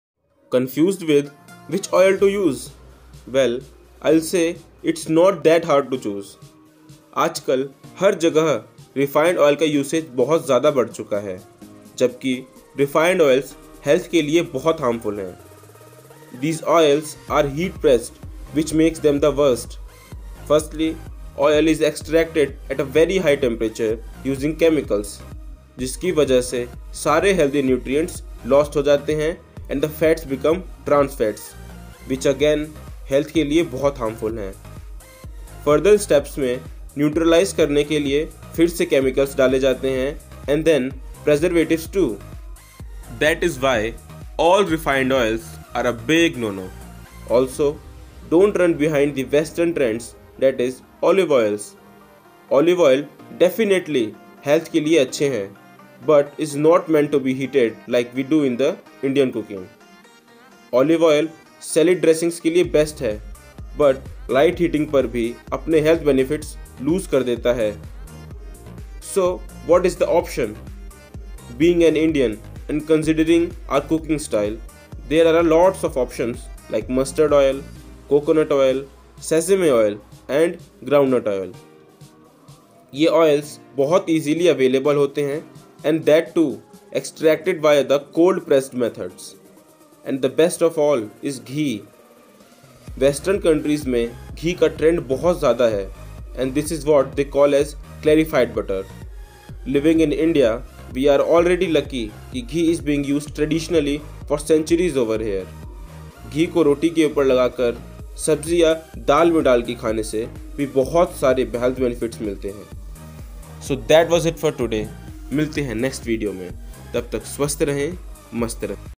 Choose the right cooking oil!!!
Confused with which cooking oil to use?
Refined oils should not be considered at all!
Olive oil is one of the healthiest oils, but is more suitable for Western countries.
If you're Indian, cold pressed oils like mustard oil, coconut oil, sesame oil and groundnut oil can be used. Ghee is also a healthy option, if used wisely. Do not get manipulated by attractive marketing, but choose what's best for you!